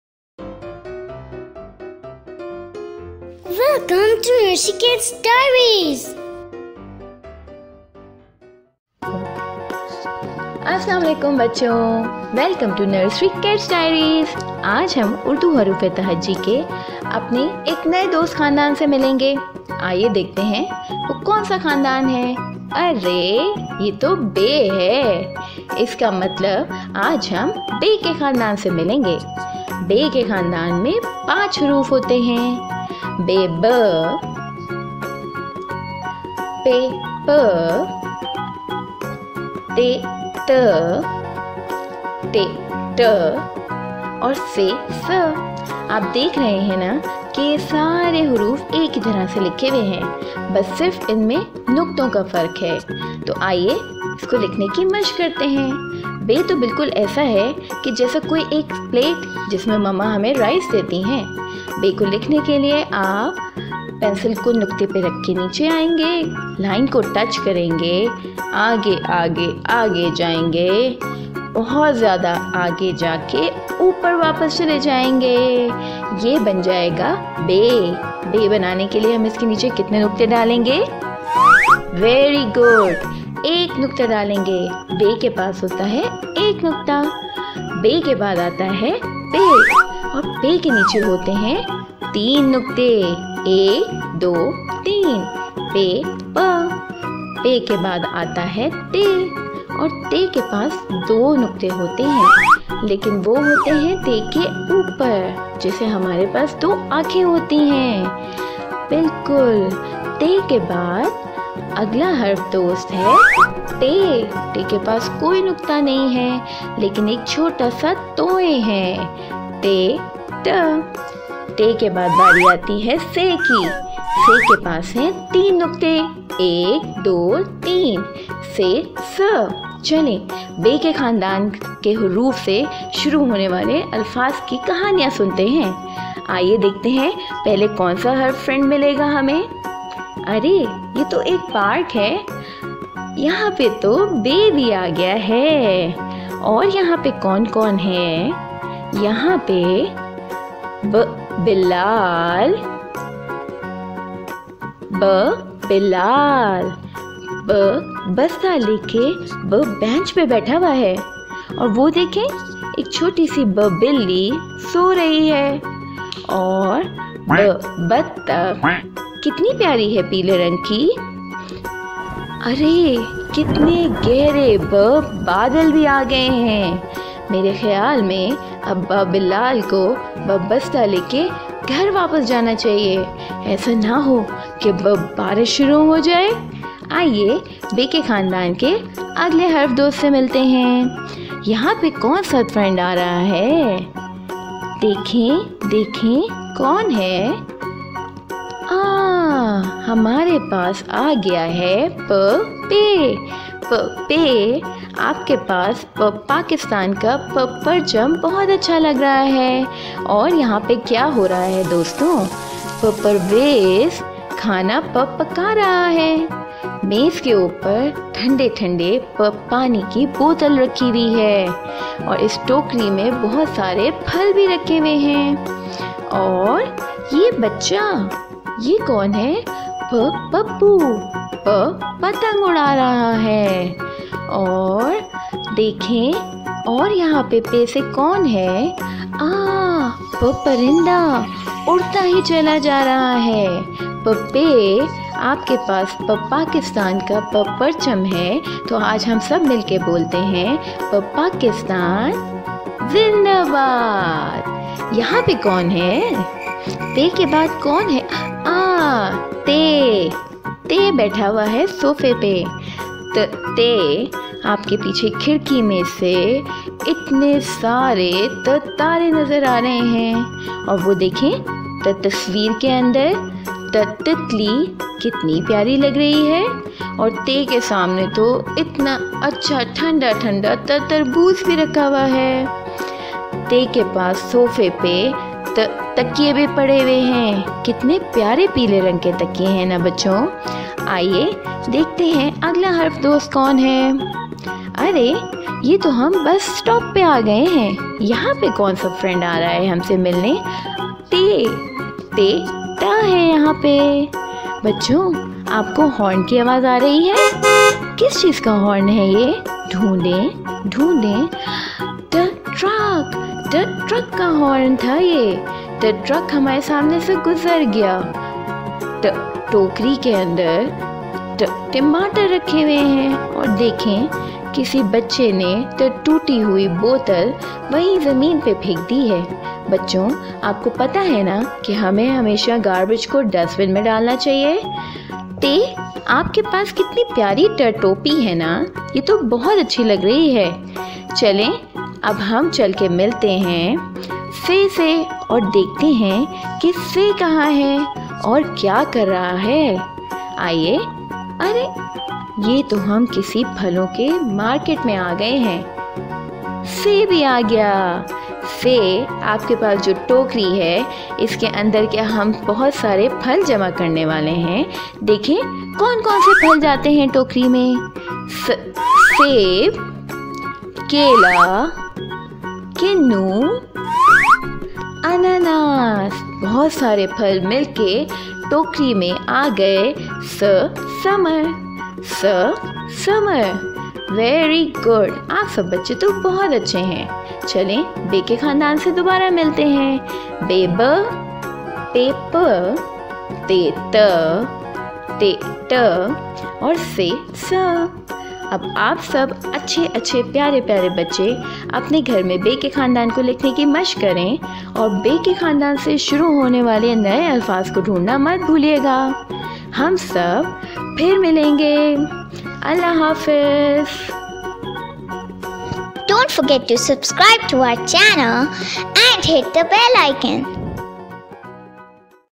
BAY KA KHANDAAN - Learning & Writing "ب پ ت ٹ ث"- Writing Urdu Haroof(Alphabets)-اردو حروفِ تحجی
Nursery KIds Diaries is a channel based upon the motive to make learning easy and fun for kids.